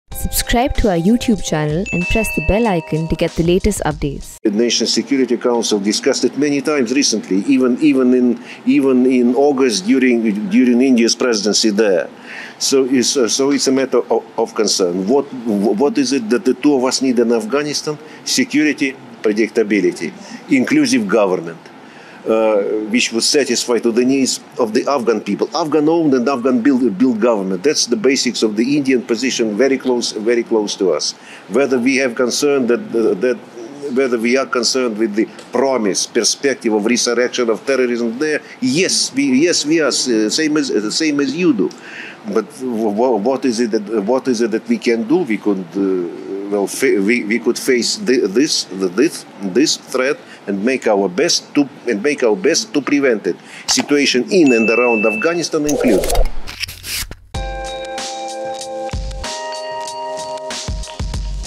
'Russia's position on Afghanistan very close to that of India'
Russian Ambassador to India Nikolay Kudashe Monday said that Russia's position on Afghanistan is "very close" to that of India and both the countries want Afghan-owned and Afghan-built government.